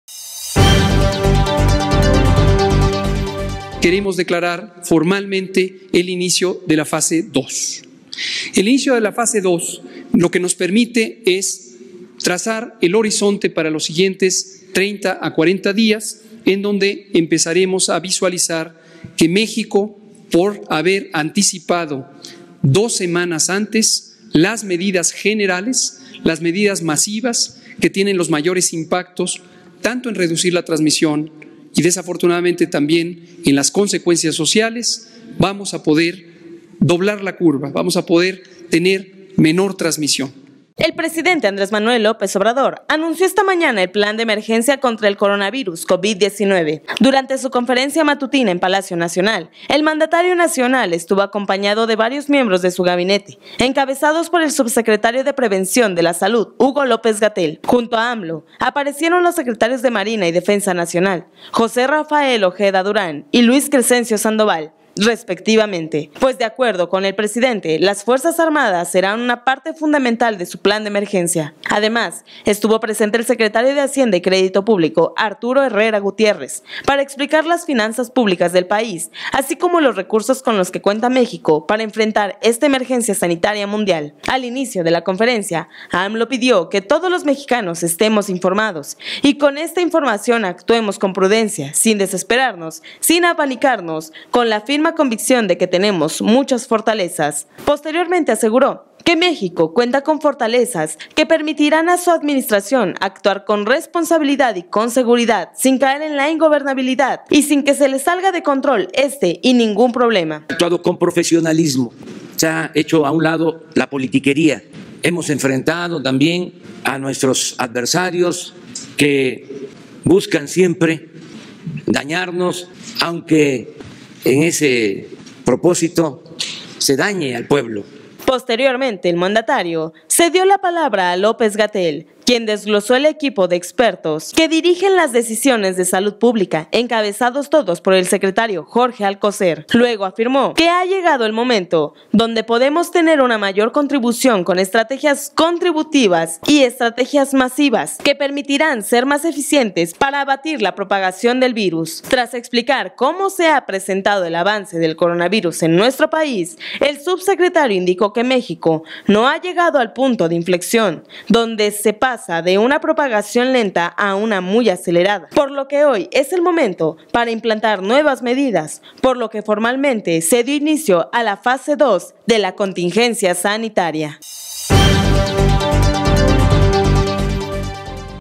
AMLO en compañía de López Gatell, presentan plan de emergencia contra Coronavirus
El presidente Andrés Manuel López Obrador (AMLO) anunció esta mañana el plan de emergencia contra el coronavirus (Covid-19).

Durante su conferencia matutina en Palacio Nacional, el mandatario nacional estuvo acompañado de varios miembros de su gabinete, encabezados por el subsecretario de Prevención de la Salud, Hugo López-Gatell.